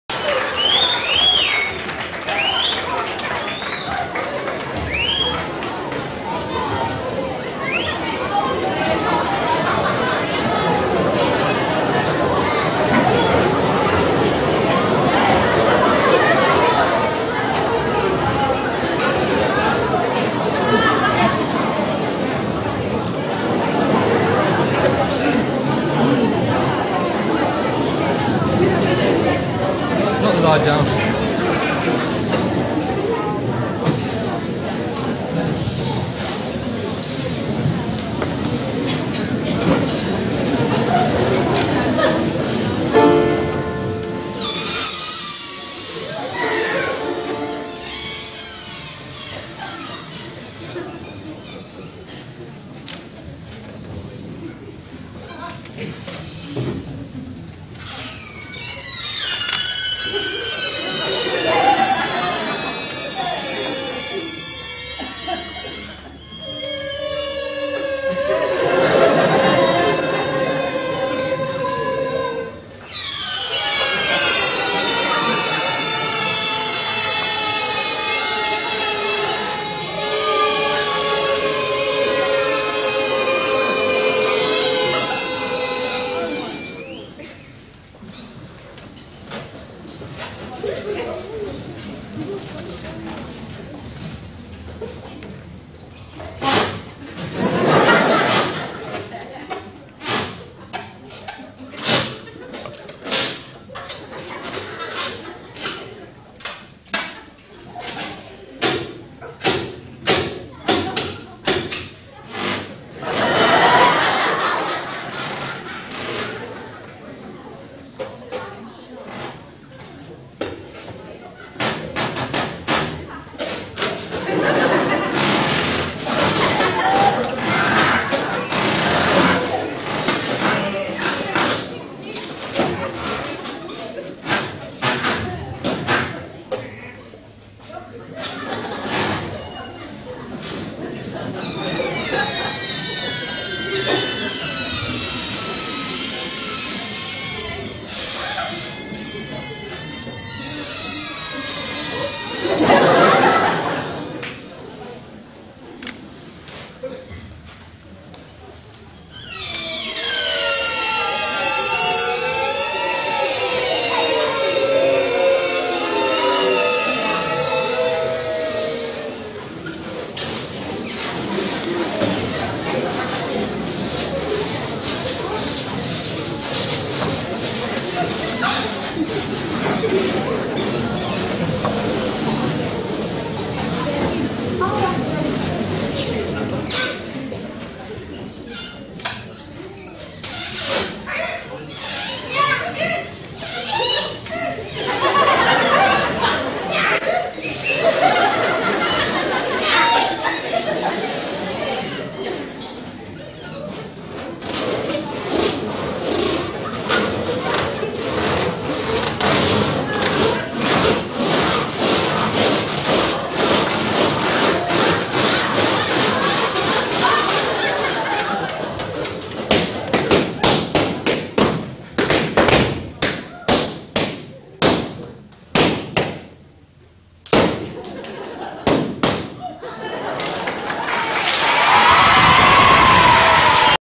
edgmusic khs staff balloon music
The Kingsbury High school staff let their hair down in this unusual performance conducted by Ruth Gibbons